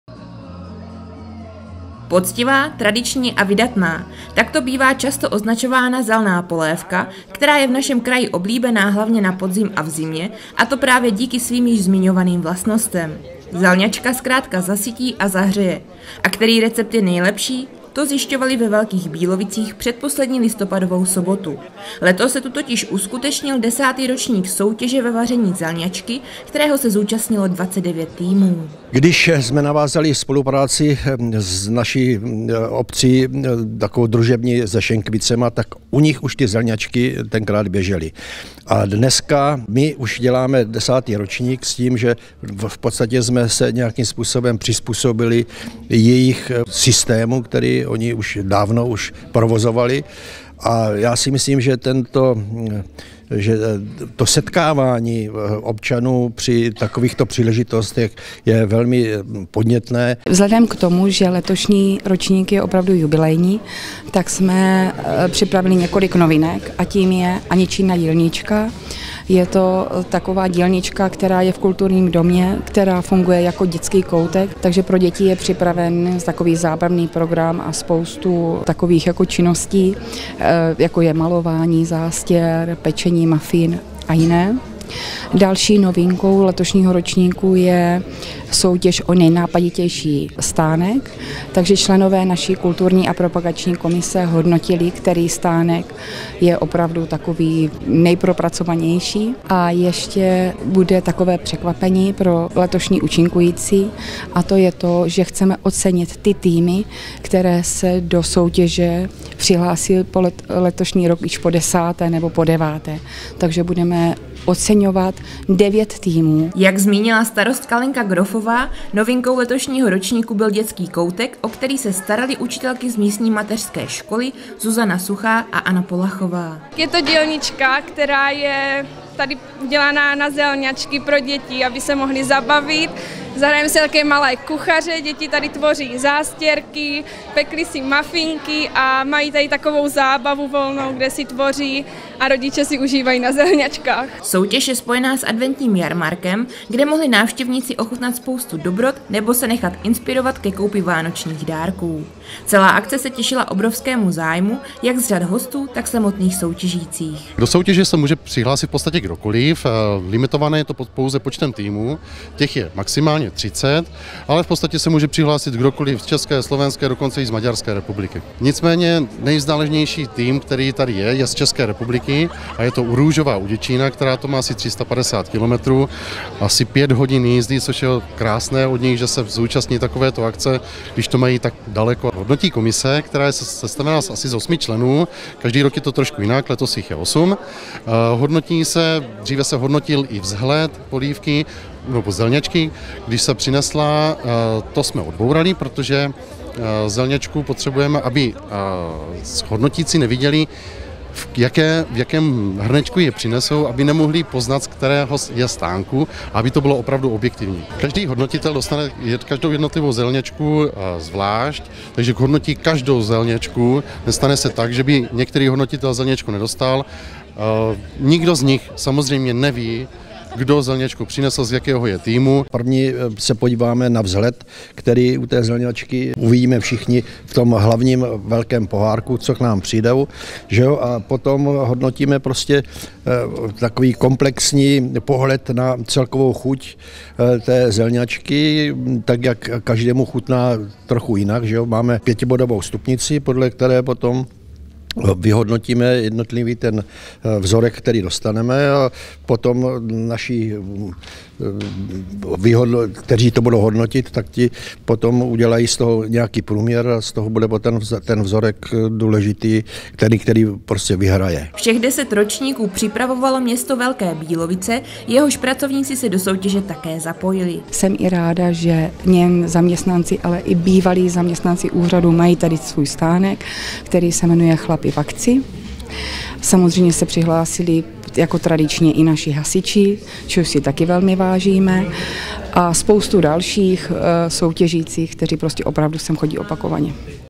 Ve Velkých Bílovicích soutěžili o nejlepší zelňačku
Poctivá, tradiční a vydatná. Takto bývá často označována zelná polévka, která je v našem kraji oblíbená hlavně na podzim a v zimě, a to právě díky svým již zmiňovaným vlastnostem. Zelňačka zkrátka zasytí a zahřeje. A který recept je nejlepší? To zjišťovali ve Velkých Bílovicích předposlední listopadovou sobotu. Letos se tu totiž uskutečnil 10. ročník soutěže ve vaření zelňačky, kterého se zúčastnilo 29 týmů.